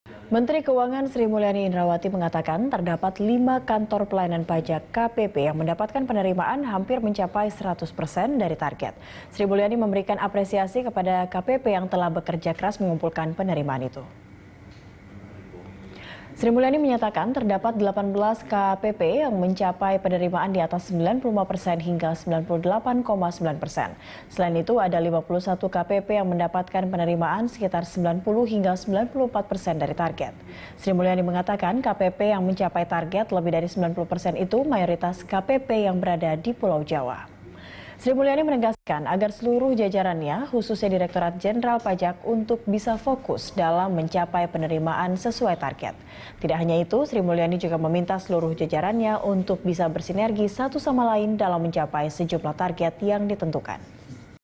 5 Kantor Pelayanan Pajak Capai 100 Persen Target Penerimaan
Menteri Keuangan Sri Mulyani Indrawati mengatakan terdapat 5 Kantor Pelayanan Pajak (KPP) mendapatkan penerimaan hampir mencapai seratus persen dari target. Sri memberi apresiasi kepada KPP yang telah bekerja keras mengumpulkan penerimaan.

Facebook.com/BeritaSatuTV
Youtube.com/BeritaSatu